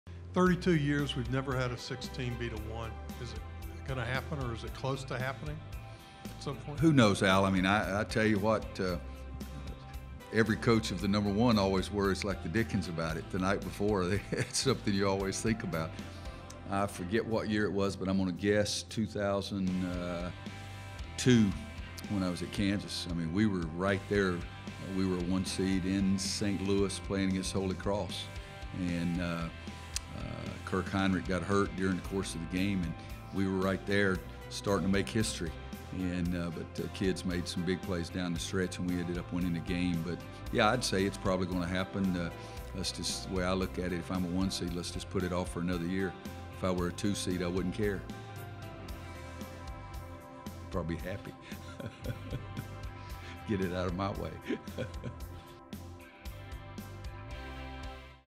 Roy Williams: 16-Seed Upsetting 1-Seed Will Happen, Hopefully Not This Year
Roy Williams is preparing his No. 1 seed Tar Heels to play against No. 16 Texas Southern.  The Hall of Fame coach was asked if he thought a 16-seed would ever upset a 1-seed.  Williams remembers a time when it almost happened while he was the coach at Kansas and says that he believes the upset will occur, just hopefully not in a year when his team is a 1-seed.